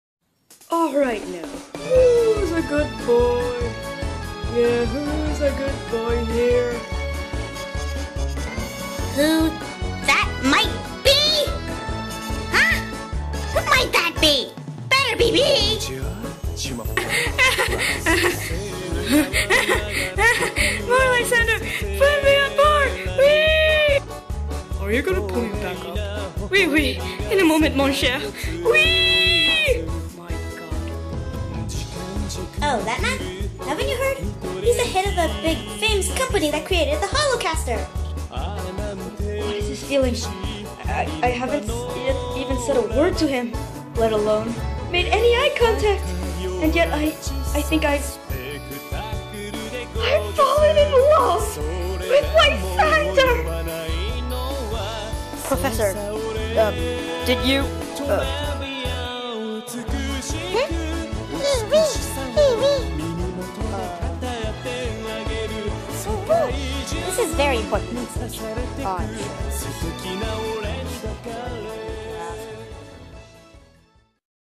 PerfectWorldShipping-comic dubs
Alright,I dont own Pokemon,I don't own the song in the background,and finally I do not own all the comics used in the video,what rightfully belongs to me is my very. very. very. very. very. VERY OBNOXIOUS VOICE. THAT IS IT. THAT IS IT.
PS I apologize if I freak you out with my voice. I know its horrible. I'm bad at doing dubs.